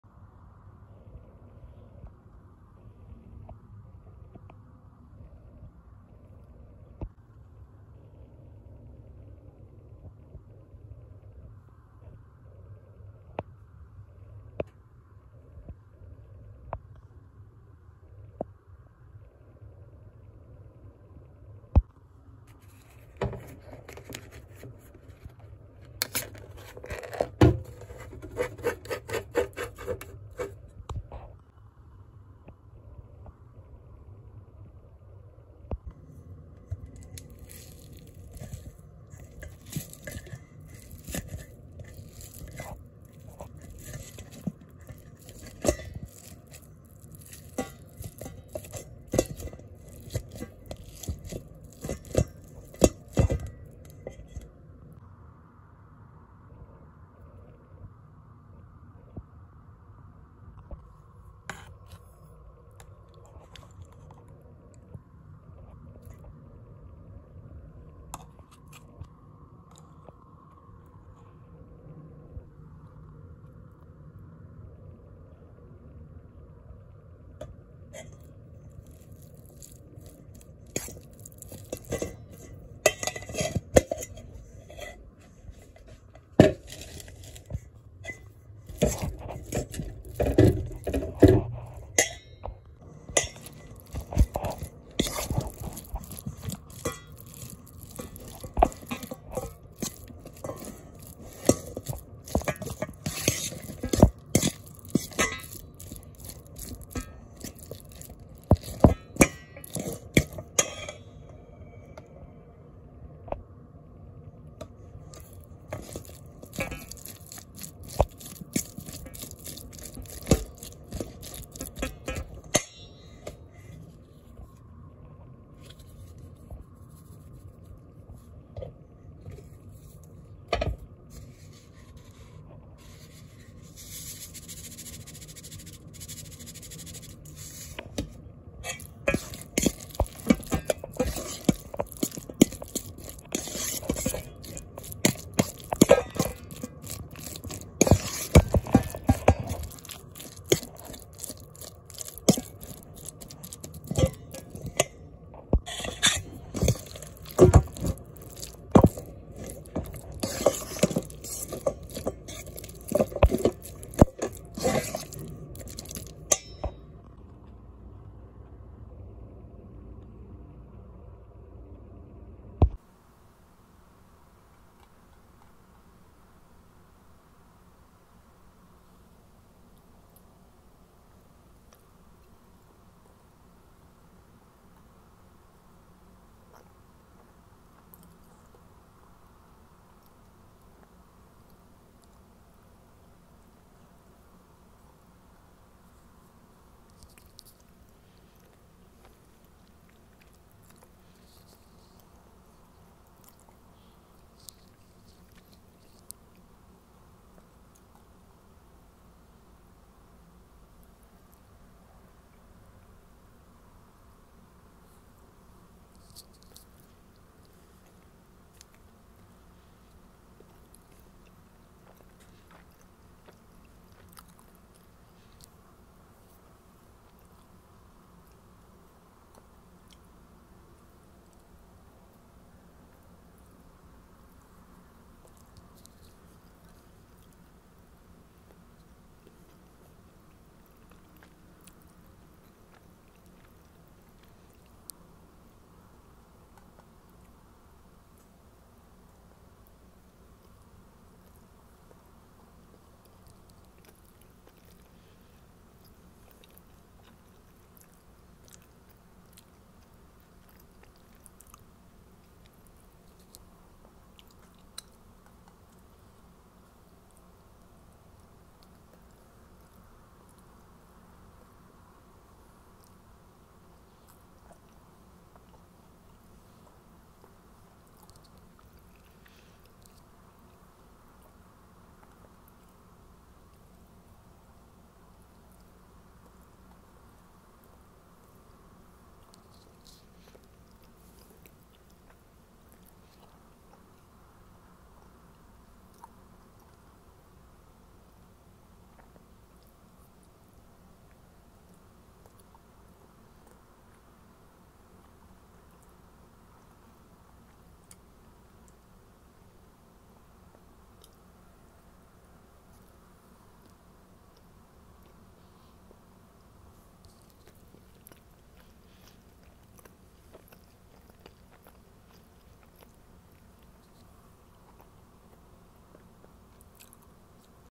Tasty 😋 Budget Chicken Sandwich with Dollarama Ingredients
In this video, we’ll show you how to make a delicious chicken sandwich on a budget using ingredients from Dollarama. We’ll use Miracle Whip, D’Italiano white bread, and canned chicken meat to create a tasty and affordable meal.